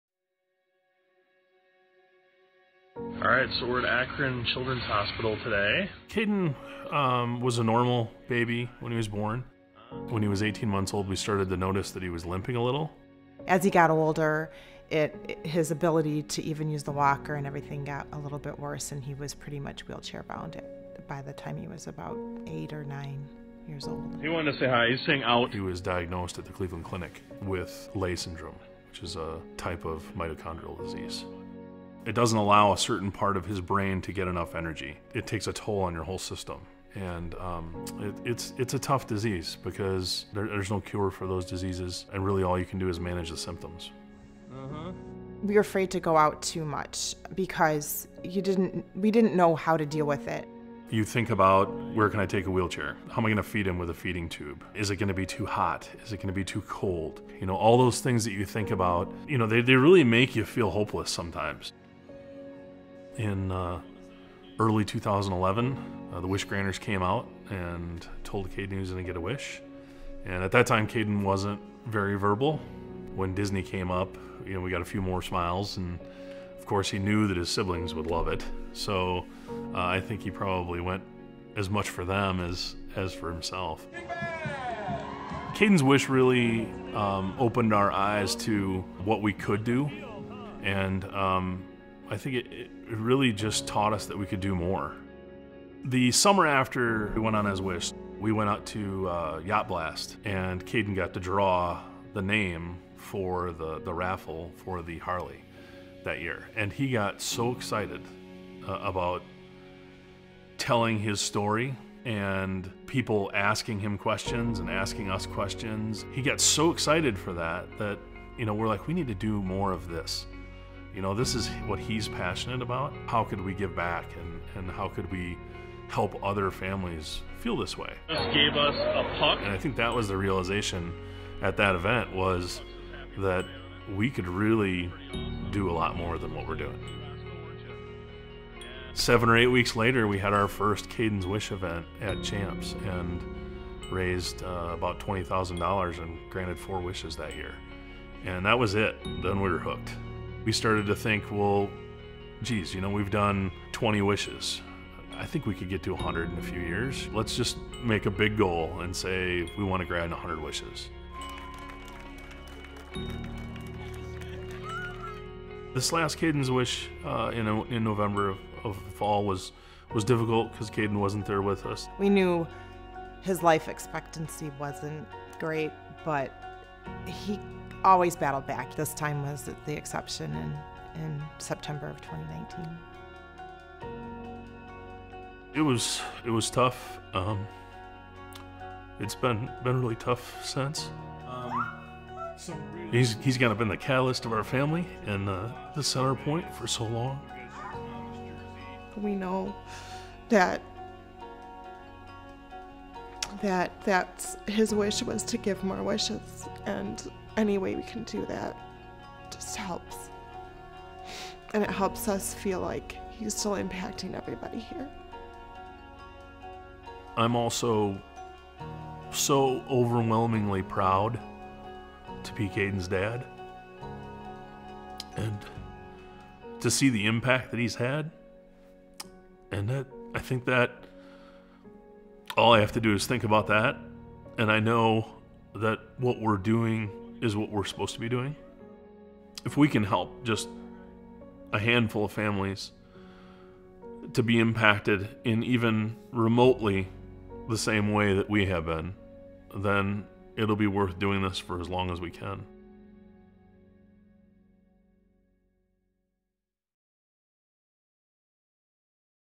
Kaden's Wish
After receiving his wish in 2011, Kaden and his family committed to raising funds to grant 100 wishes. Kaden was presented with the WISH Award at this year's Wish Night, a Virtual Gala. While Kaden is no longer with us, the impact of his life and legacy continue on.